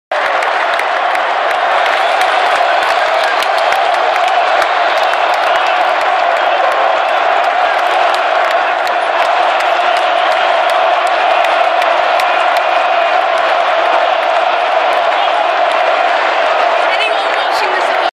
5-5 Boing boing celebration
West Brom's celebration for the 5th goal vs Man Utd